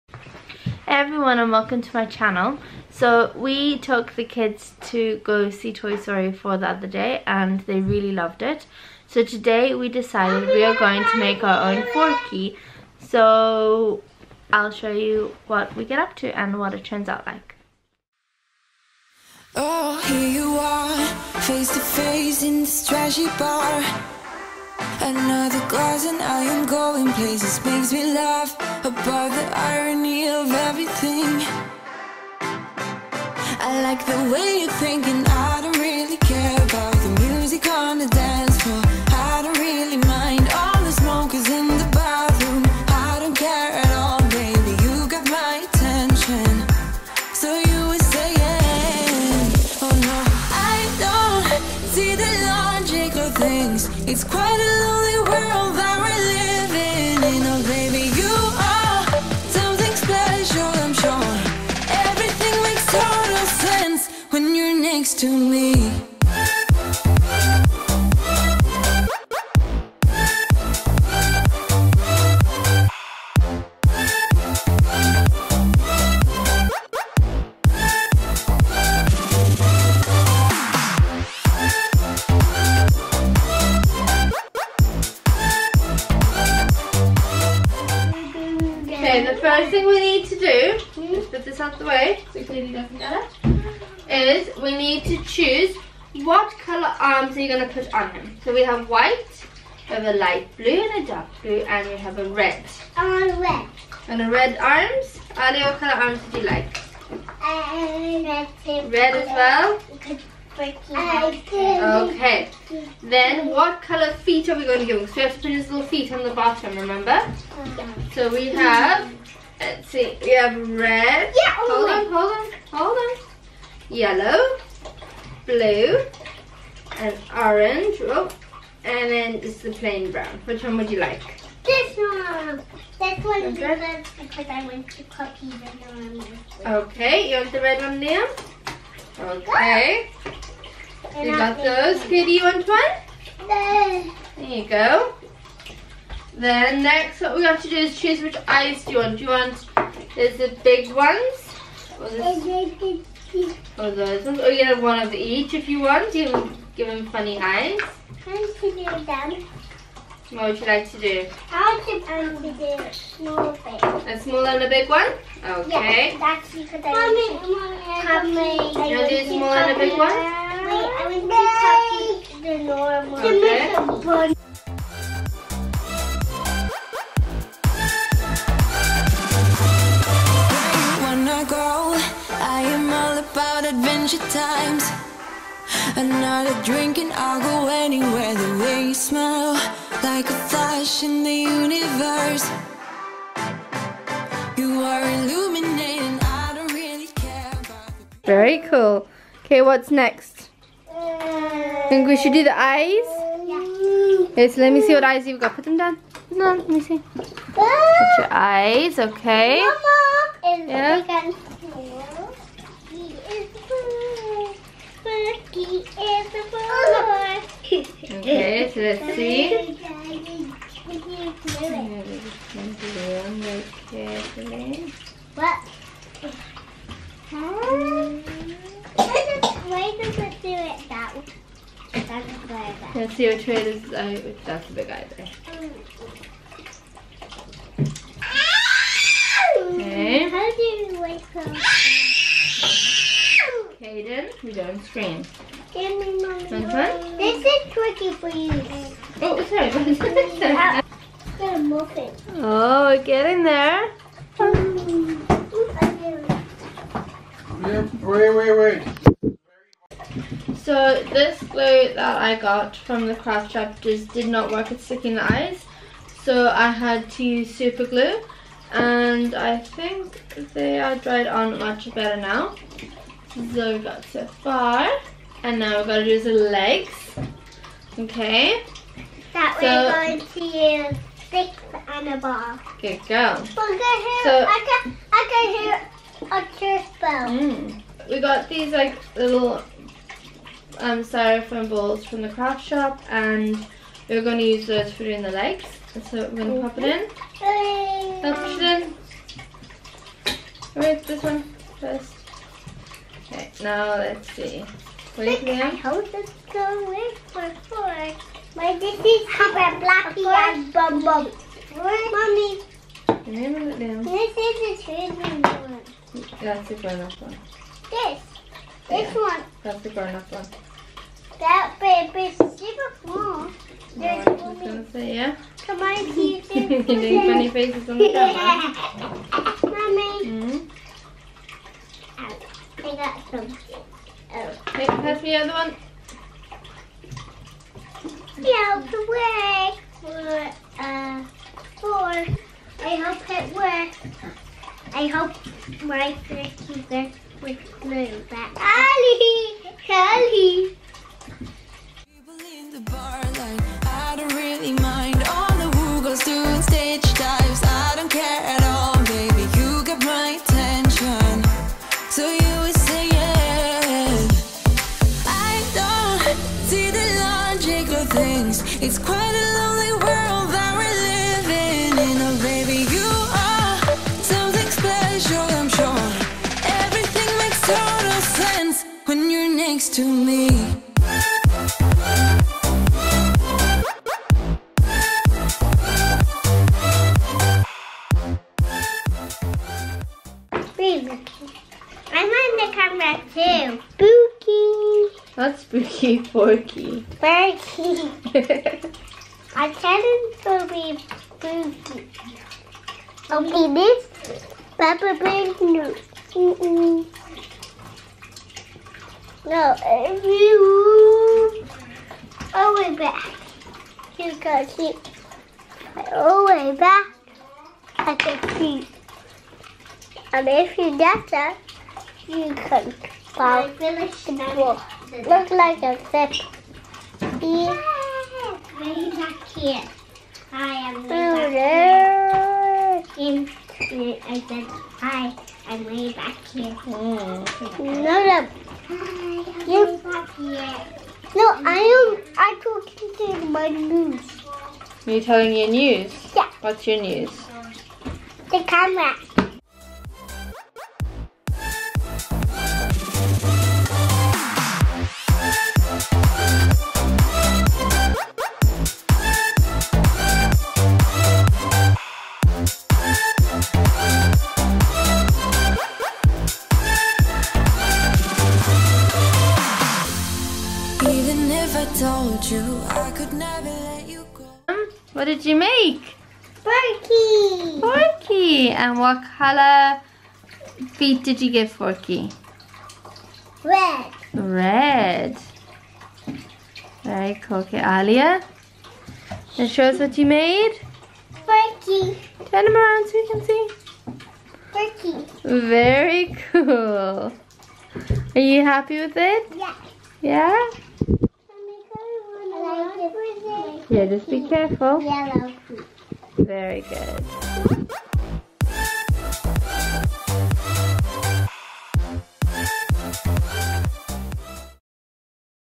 Today we made Forky // Toy Story 4 // Forky
My kids really loved the new movie Toy Story 4, so we decided to make out own Forky. Something easy and fun to make with your kids.